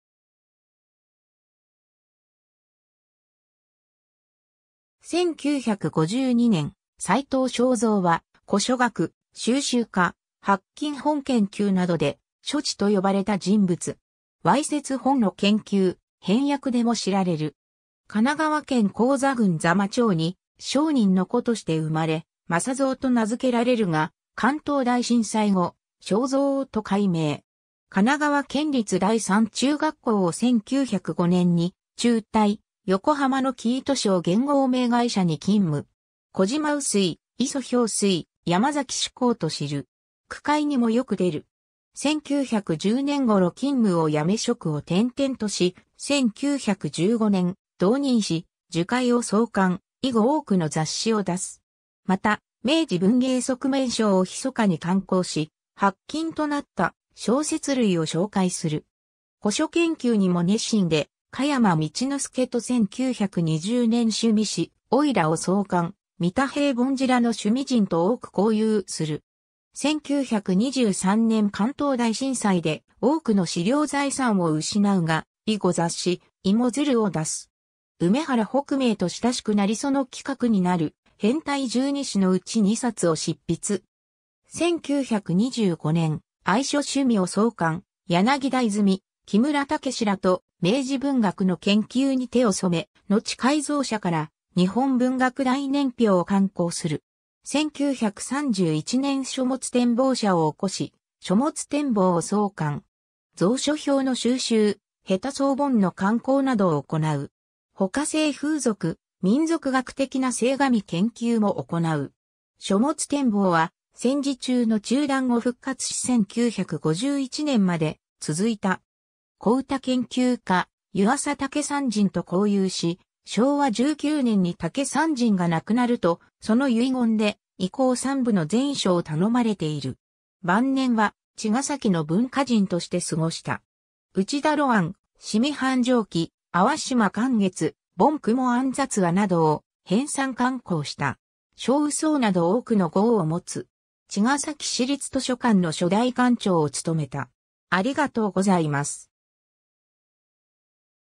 斎藤昌三 (古書研究家)
1952年 斎藤 昌三（さいとう しょうぞう、1887年3月19日 - 1961年11月26日）は、古書学、蒐集家、発禁本研究などで「書痴」と呼ばれた人物。
猥褻本の研究、編訳でも知られる。
 神奈川県高座郡座間町（現・座間市）に商人の子として生まれ、政三と名づけられるが、関東大震災後「昌三」と改名。
 神奈川県立第三中学校（現・神奈川県立厚木高等学校）を1905年に中退、横浜の生糸商原合名会社に勤務。
小島烏水、礒萍水・山崎紫紅と知る。
句会にもよく出る。
1910年頃勤務をやめ職を転々とし、1915年同人誌『樹海』を創刊、以後多くの雑誌を出す。
また『明治文藝側面鈔』を密かに刊行し、発禁となった小説類を紹介する。
 古書研究にも熱心で、加山道之助と1920年趣味誌『おいら』を創刊、三田平凡寺らの趣味人と多く交友する。
1923年関東大震災で多くの資料財産を失うが、以後雑誌『いもづる』を出す。
梅原北明と親しくなりその企画になる「変態十二史」のうち二冊を執筆。
1925年『愛書趣味』を創刊、柳田泉、木村毅らと明治文学の研究に手を染め、のち改造社から『日本文学大年表』を刊行する。
 1931年書物展望社を興し『書物展望』を創刊。
蔵書票の蒐集、下手装本の刊行などを行う。
ほか性風俗、民俗学的な性神研究も行う。
『書物展望』は戦時中の中断後復活し1951年まで続いた。
小唄研究家・湯朝竹山人と交遊し、昭和19年に竹山人が亡くなるとその遺言で遺稿3部の善処を頼まれている。
 晩年は茅ヶ崎の文化人として過ごした。
内田魯庵『紙魚繁盛記』、淡島寒月『梵雲庵雑話』などを編纂刊行した。
少雨叟など多くの号をもつ。
 茅ケ崎市立図書館の初代館長を務めた。